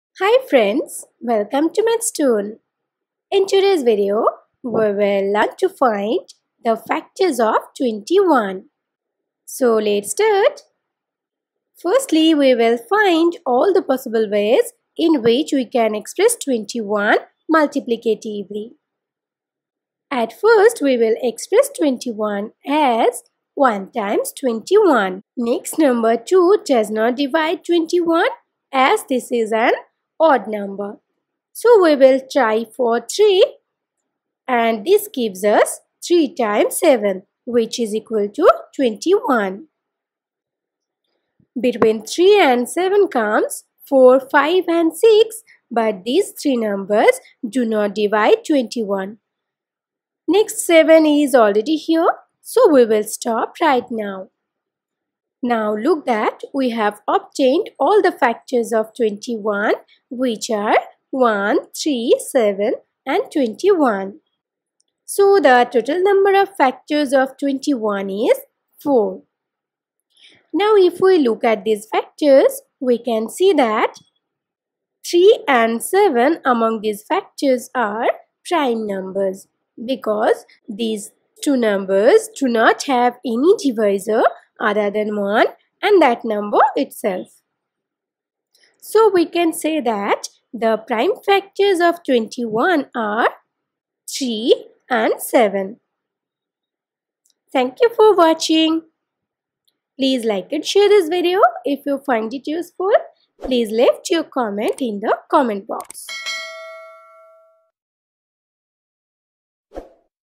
Factors of 21 | Prime factors of 21
In this video, we will find the factors of 21 in a very easy way.
Topic: Factor of 21 | Prime factor of 21

Question: What are the factors of 21?
Answer: The factors of 21 are 1, 3, 7, 21.
So the total number/count of factors of 21 is four.

Question: What are the prime factors of 21?
Answer: The prime factors of 21 are 3 and 7.

Chapters:
0:00 - Introduction
0:11 - Solution of what are the factors of 21
0:55 - List of factors of 21
1:10 - Prime factors of 21
1:40 - Like, comment, share the video

We post videos daily on youtube